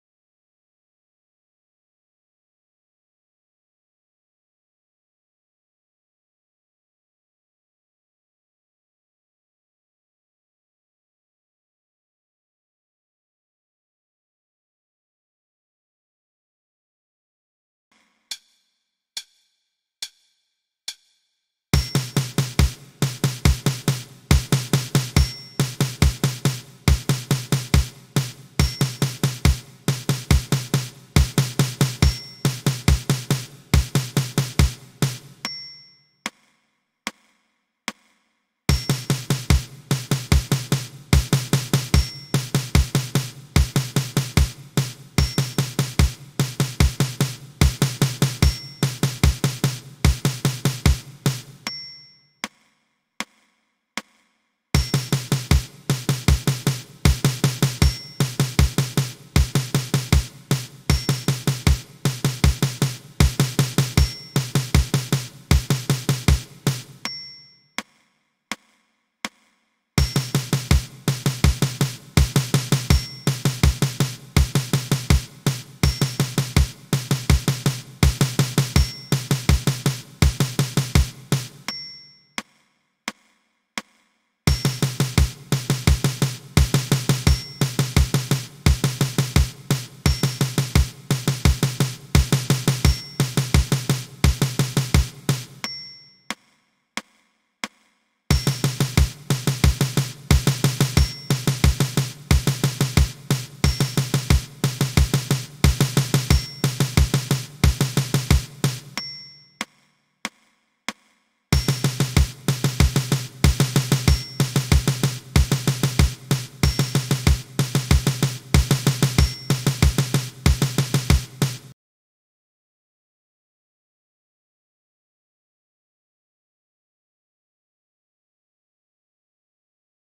Funky Primer page 11, exercise 20 Drum play-along with w/ specified sticking RLRRL
This is a 4 measure exercise with one-measure rest.
The tempo will increase and the exercises will play again.
Read from your book or follow the cursor to play along.
Try adjusting the playback speed (in YouTube settings) to increase or decrease difficulty.